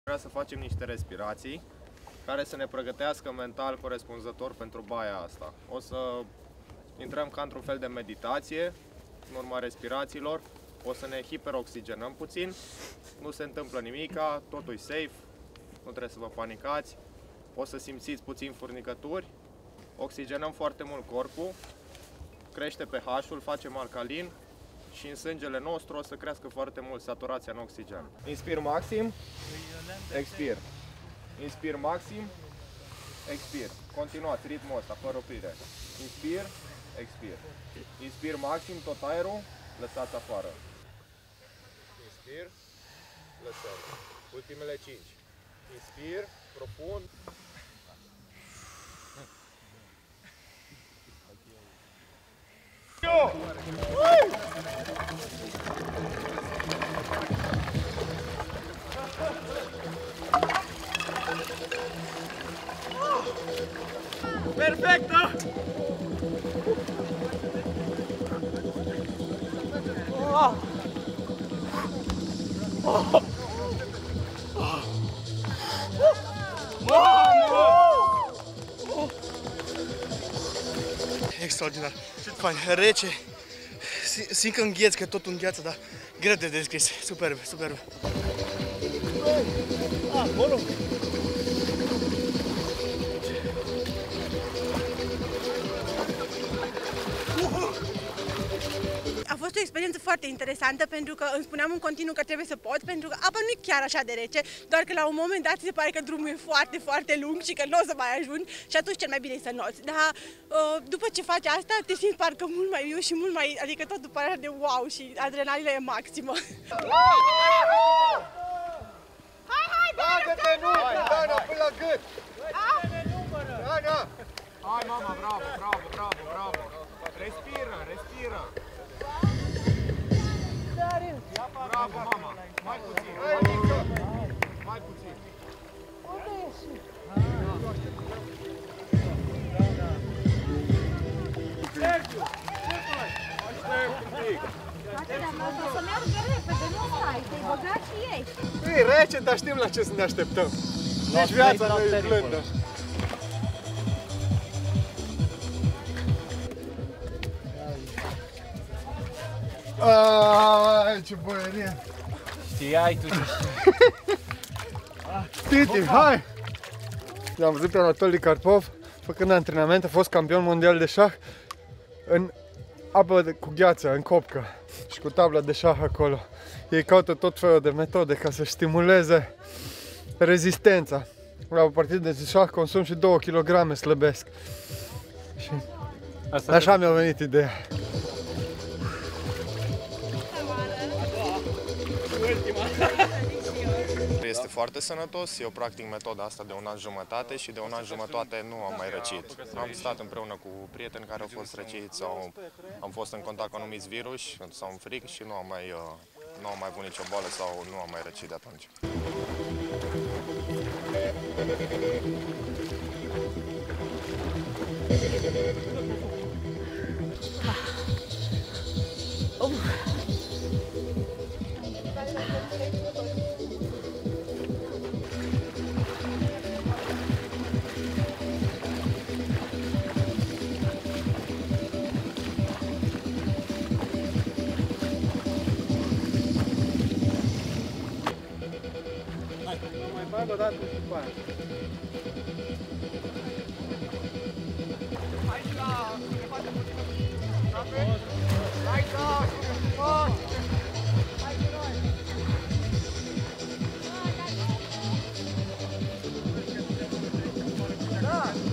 Baie la copca langa Timisoara
Baie la copca langa Timisoara. 20 de temerari s-au avantat in apele reci ca gheata. Imaginile distractiei la -6 grade Celsius. Video